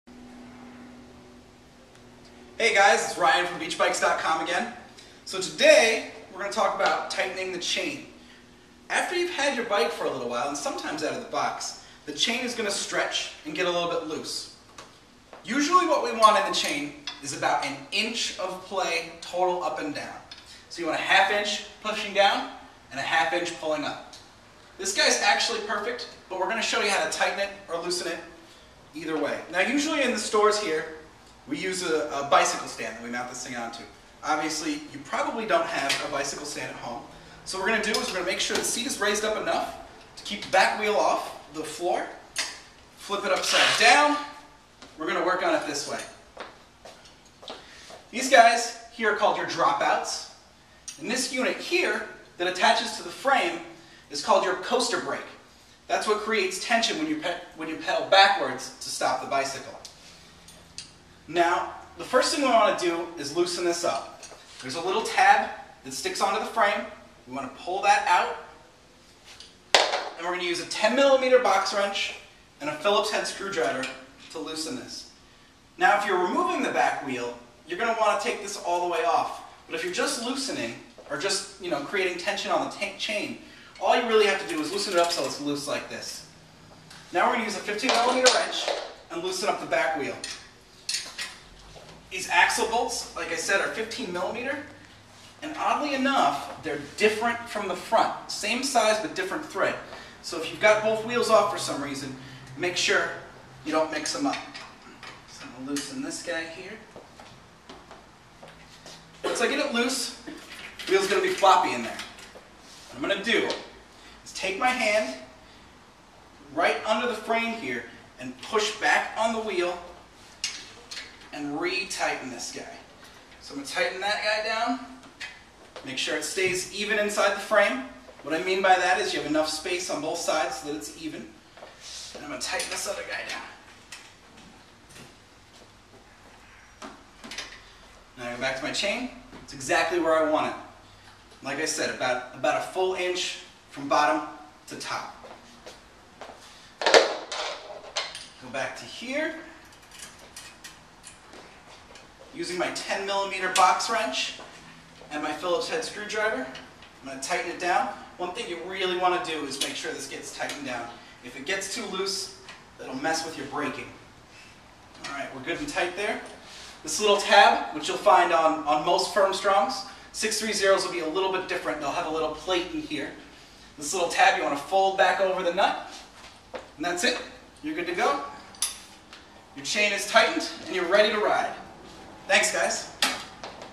Tightening the chain on a beach cruiser bike - Beachbikes
Ryan from Beachbikes.net shows us how to tighten the chain on beach cruiser bicycle.  Sometimes direct from the factory your chain may be not quite tight enough.  Also, over time as the chain stretches it will loosen and need to be tightened.  Ryan explains how to do it.